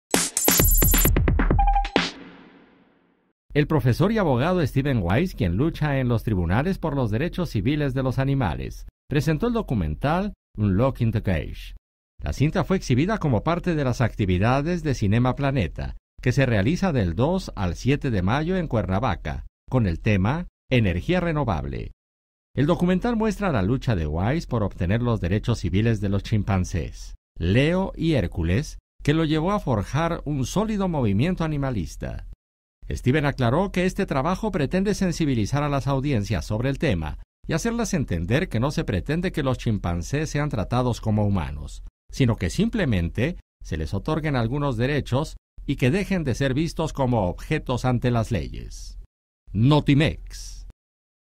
Presentan documental sobre derechos civiles de los Chimpancés
El profesor y abogado, Steven Wise, quien lucha en los tribunales por los derechos civiles para los animales, especialmente para los chimpancés, presentó el documental “Unlocking the cage” en Cienama Planeta